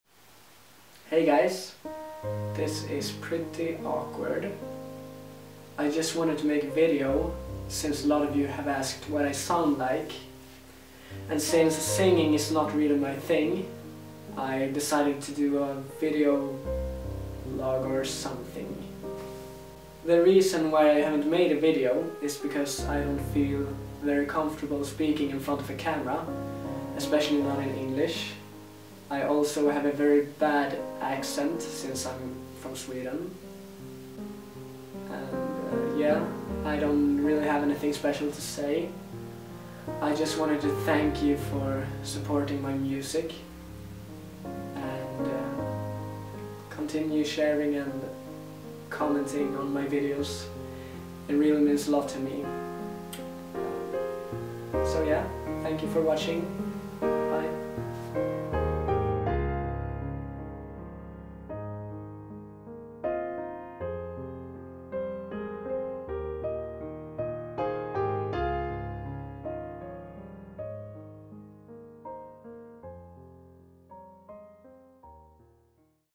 Video Update 1 - Awkwardness
I've never made a video like this before, it's hard for me since I don't feel comfortable at all with it, I also don't like the sound of my own voice and my accent which makes it even harder. I might take it down or something, I dunno. Hope you enjoy it though, sorry for the awkwardness...

The song you hear is a yet unreleased song by me.